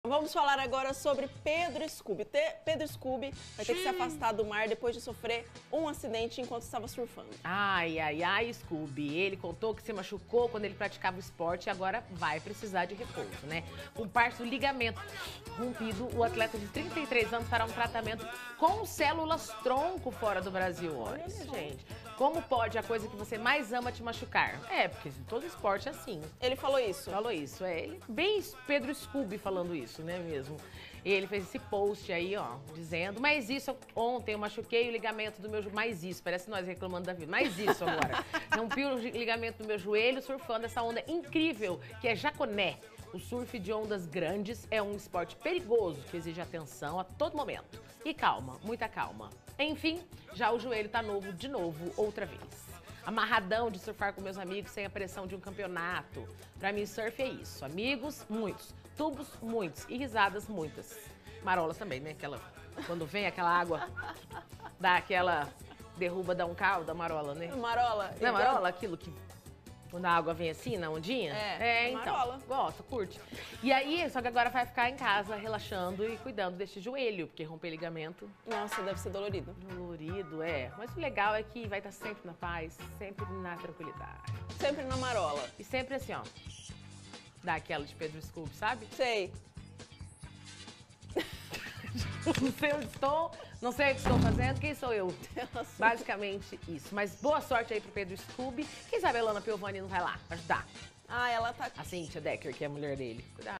Pedro Scooby sofre acidente surfando
Pedro Scooby fará tratamento com células-tronco após se machucar surfando e romper ligamentos do joelho.